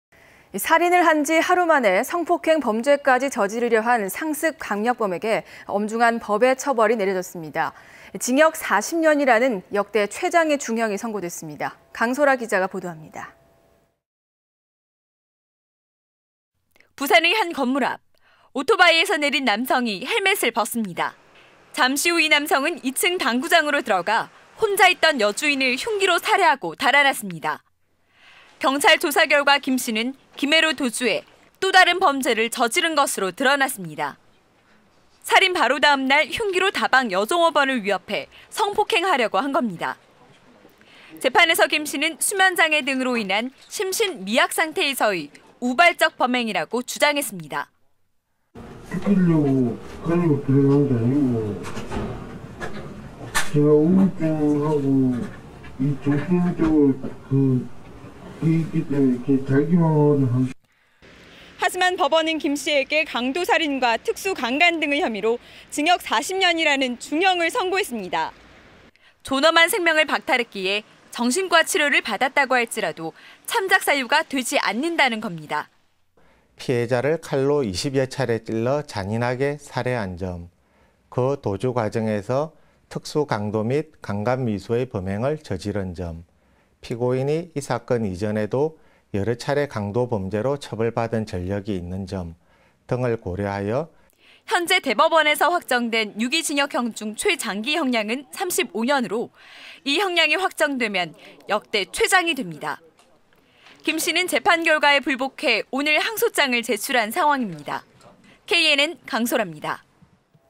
[KNN 뉴스] 뻔뻔한 살인범에 역대 최장 중형
{앵커:
살인을 한 지 하루만에 성폭행 범죄까지 저지르려한 상습 강력범에게
엄중한 법의 처벌이 내려졌습니다.

징역 40년이라는 역대 최장의 중형이
선고됐습니다.

강소라 기자가 보도합니다.}

{리포트}
{수퍼:살인사건 현장 CCTV / 지난해 10월 21일, 부산 동대신동}
부산의 한 건물앞.

오토바이에서 내린 남성이
헬맷을 벗습니다.

잠시 후 이 남성은 2층 당구장으로
들어가 혼자있던 여주인을 흉기로
살해하고 달아났습니다.

{수퍼:살인한 흉기로 또 범행/다방 여종업원 성폭행미수}
경찰 조사결과 김 씨는 김해로
도주해 또 다른 범죄를 저지른 것으로 드러났습니다.

살인 바로 다음 날, 흉기로 다방
여종업원을 위협해 성폭행하려고
한겁니다.

{수퍼:'심신미약 상태임을 주장'}
재판에서 김 씨는 수면장애 등으로
인한 심신미약 상태에서의 우발적
범행이라고 주장했습니다.

{싱크:}
{수퍼:김모 씨/범인 "죽이려고 (칼을) 가지고 들어간게 아니고, 제가 우울증하고, 정신 쪽으로 문제가 있기 때문에 자기방어를 (위해서 갖고 있었습니다.)"}

{StandUp}
{수퍼:강소라}
"하지만 법원은 김씨에게 강도살인과 특수강간 등의 혐의로
징역 40년이라는 중형을 선고했습니다."

존엄한 생명을 박탈했기에
정신과 치료를 받았다고 할지라도
참작사유가 되지 않는다는 것입니다.

{인터뷰:}
{수퍼:조민석/부산지방법원 부장판사 "피해자를 칼로 20여 차례 찔러 잔인하게 살해한 점, 그 도주 과정에서 특수강도 및 강간미수의 범행을 저지른 점, 피고인이 이 사건 이전에도 여러차례 강도범죄로 처벌받은 전력이 있는 점 등을 고려해 (선고했습니다.)"}

{수퍼:영상취재 박동명*신동희*이원주}
{수퍼:영상제공 부산 서부경찰서}

현재 대법원에서 확정된 유기징역형 중 최장기 형량은 35년으로 이 형량이 확정되면 역대 최장이 됩니다.

김씨는 재판결과에 불복해 오늘(12)
항소장을 제출한 상황입니다.

KNN 강소라입니다.